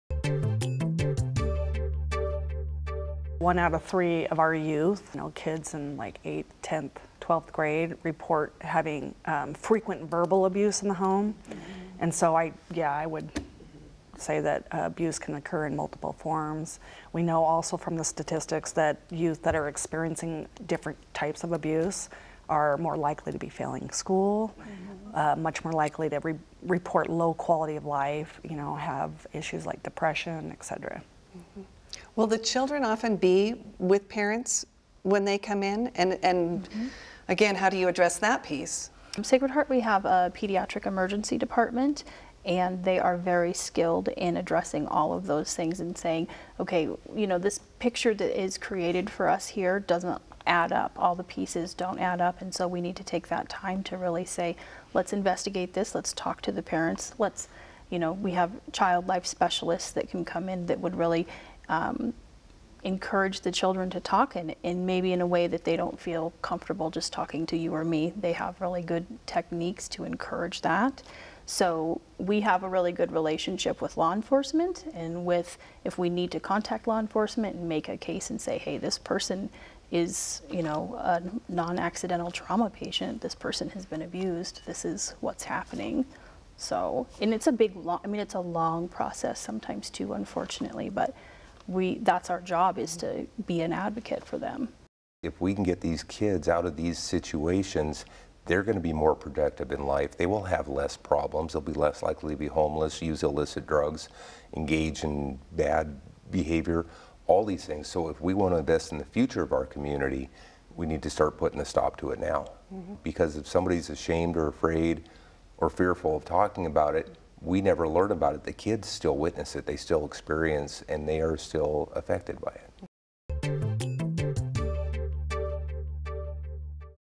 Helping Children in an Abusive Home
Child abuse takes many forms. Children who suffer frequent verbal abuse in the home and witness domestic violence are effected emotionally and are likely to develop behavioral problems.

Health Matters panelist Stacy Wenzl of Spokane Regional Health District; Leah Edwards, RN of Providence Sacred Heart Medical Center, and Sgt. Jordan Ferguson of Spokane Police Department discuss.